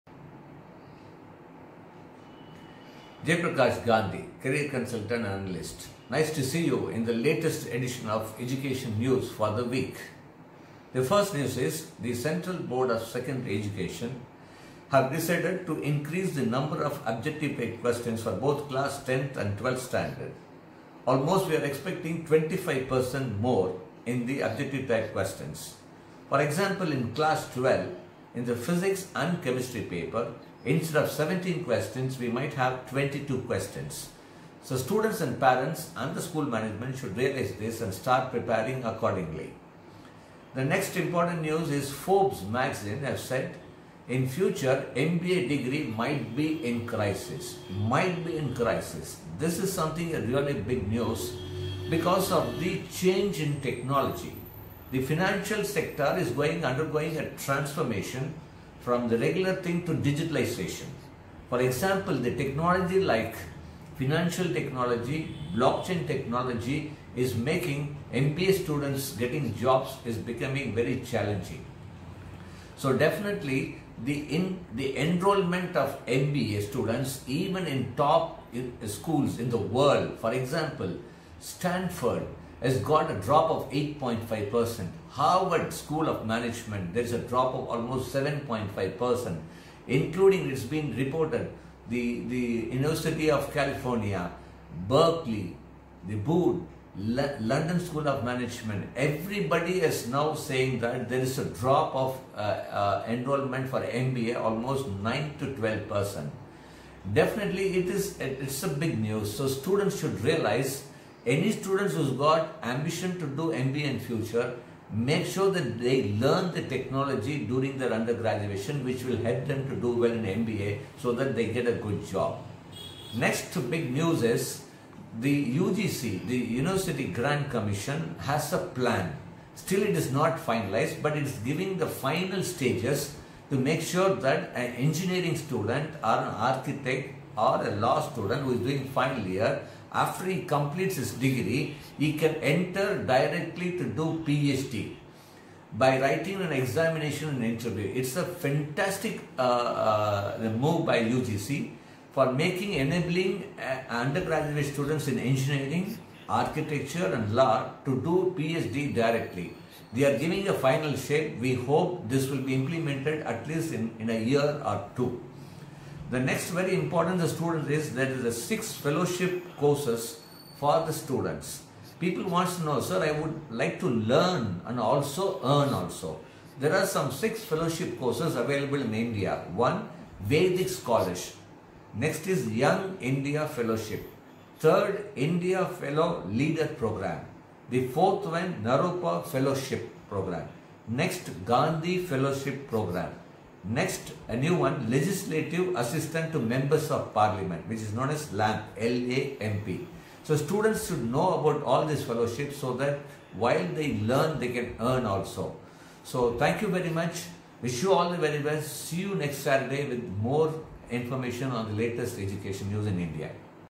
Latest Education News and Updates of the Week - For Students & Parents | 6
To all the students and parents, did you'll miss the news and updates in the education sector last week? what are the new competitive exams, deadlines, updates on policy etc.
Watch Career Guidance and Education Specialist explain in detail about the Education News of the Week.

By Jayaprakash Gandhi - Career Guidance and analyst for over 19 years and counseled over 1.75 million students across the world.